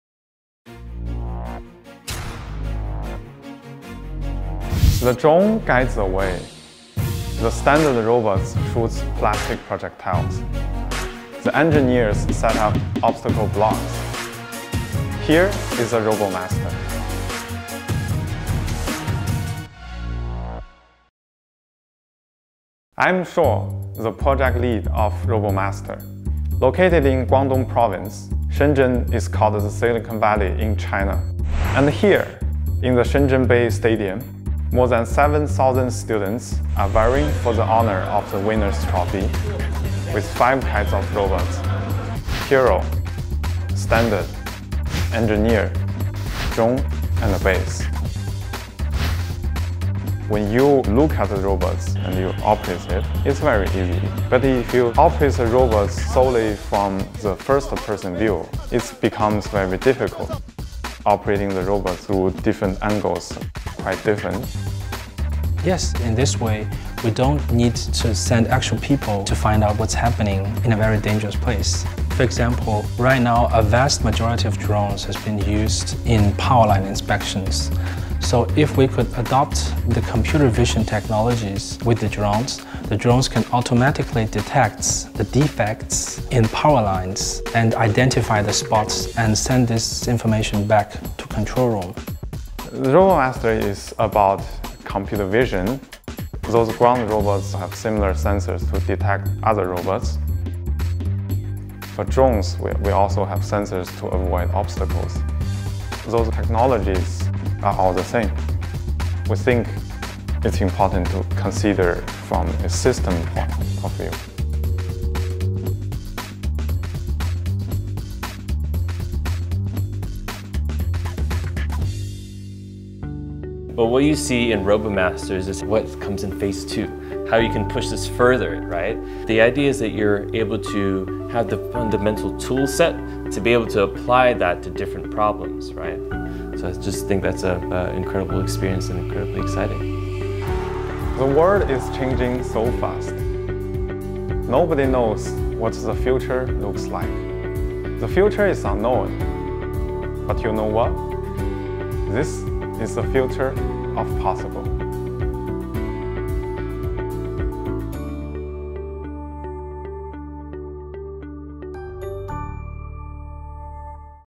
(ENG) DJI Robomaster 2017: The Future Of Possible
"Technology changes the world. This is the future of possible"
Shenzhen, China's Silicon Valley, is where the story of geeks creating the future begins.

r u ready to INSPIRE💙?